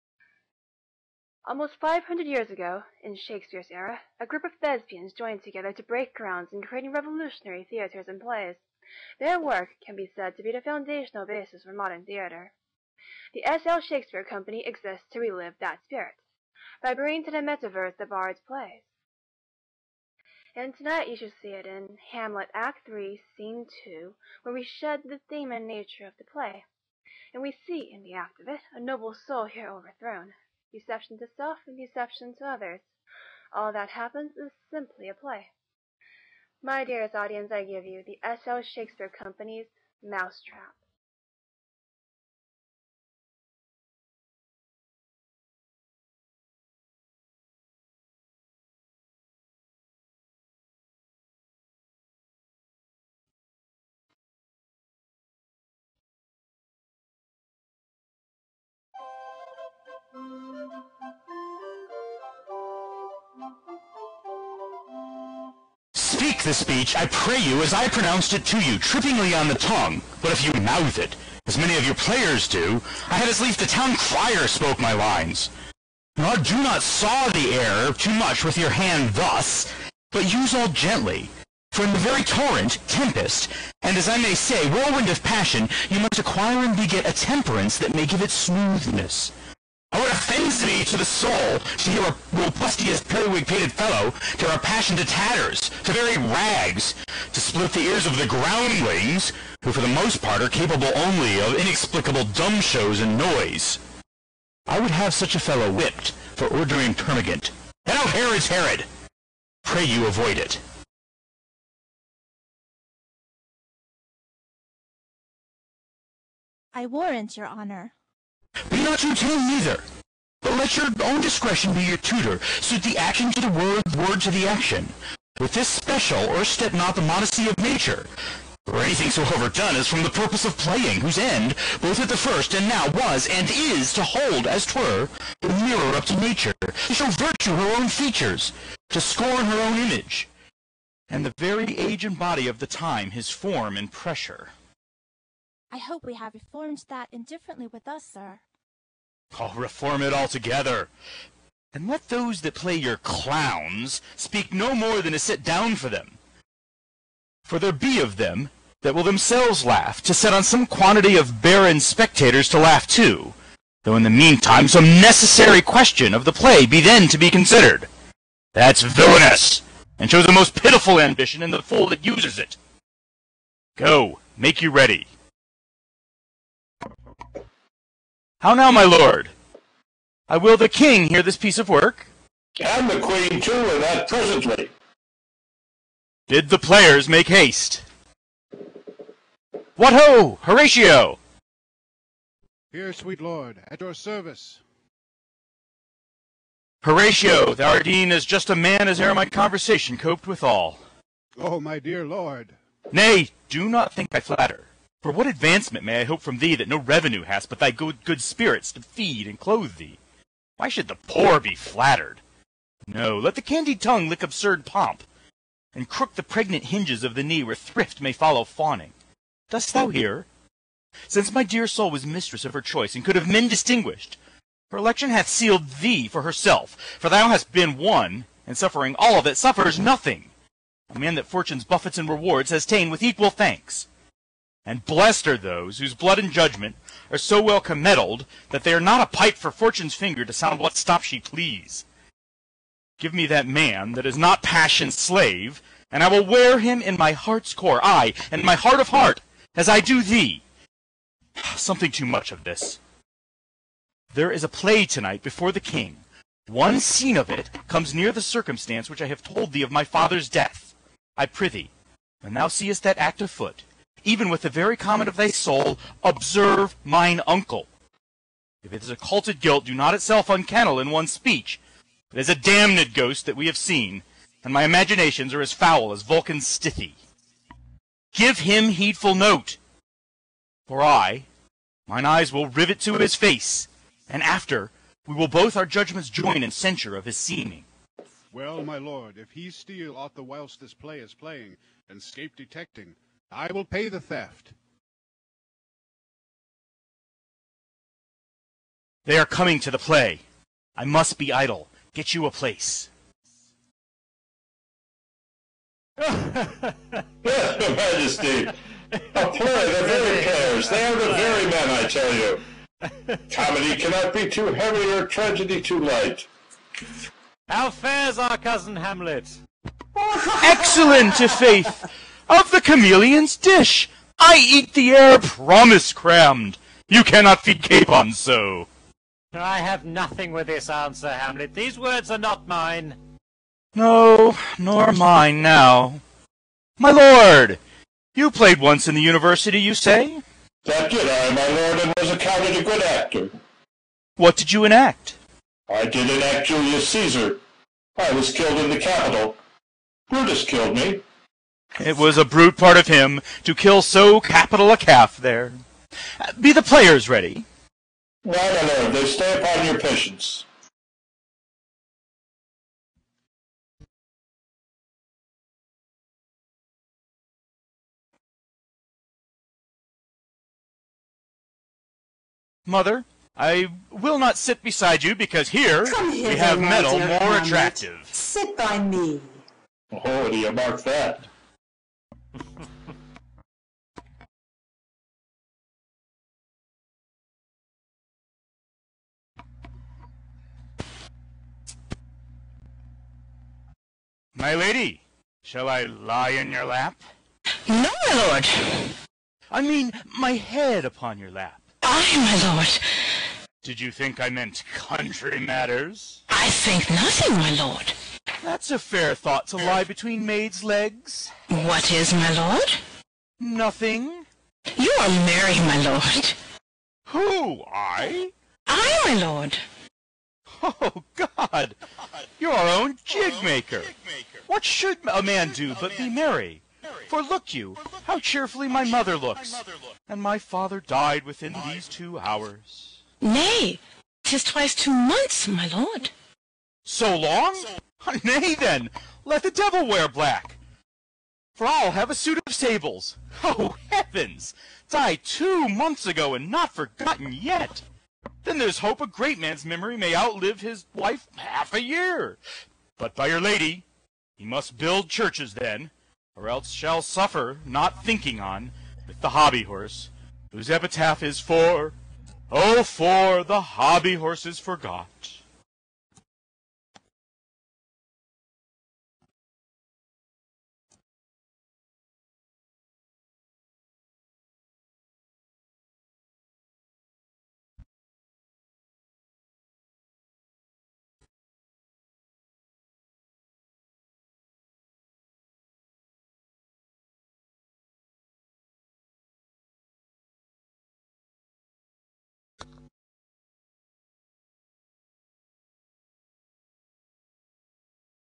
SL Shakespeare Company - Hamlet
On the birthday of the big fella, the Second Life Shakespeare company performed Hamlet LIVE. Here is a snip. Interesting for Educators, as it gives a new option for delivering assignments and drama.